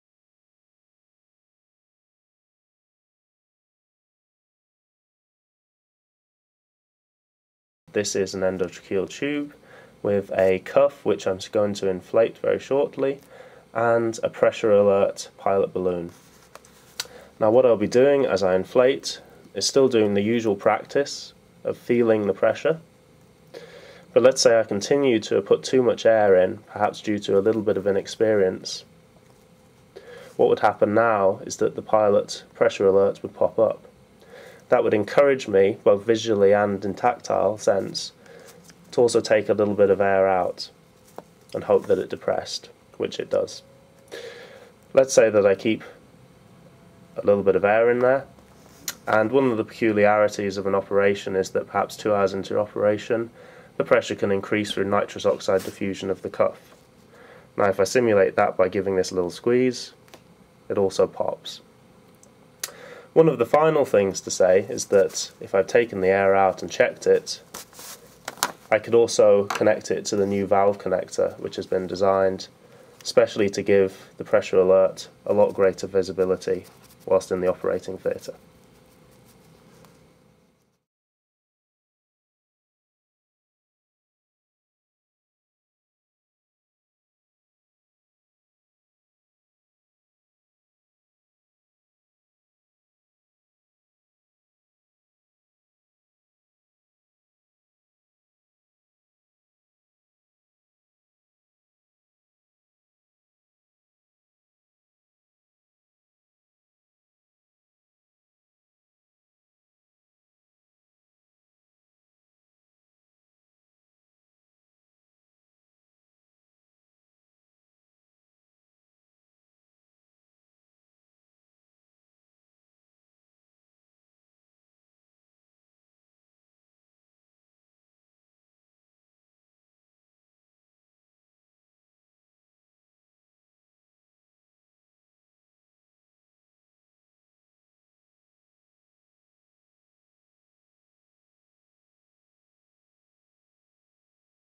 Pressure Alert for an Endotracheal Tube
The Pressure Alert is a unique adaptation for a medical device known as an Endotracheal Tube.  To prepare for an operation, the tube is inserted into the patients airway to allow them breathe. The upper airway is then sealed to prevent leaks by inflating a small balloon called a Cuff. The pressure of this cuff is critical; too low would cause leaks resulting in the patient receiving too little gas; too high and the Cuff balloon could bruise or in worst cases split the trachea. Currently, the way to judge this safe pressure is by feeling (squeezing) a similar Pilot balloon, outside the body. As with many tactile senses, accuracy comes with time and practice, but even seasoned anesthetists can misjudge this delicate process.

Pressure Alert is the first device that helps prevent trauma by giving a pop-up warning integrated into the pilot balloon itself, to alert the user that pressure is too high, both at the start and during the course of the operation.

The device has been enthusiastically received by a wide range of medical practitioners: An excellent conceptnice and simple, easy to see, no extra connections and was considered to be a significant step forward in patient care if successfully launched.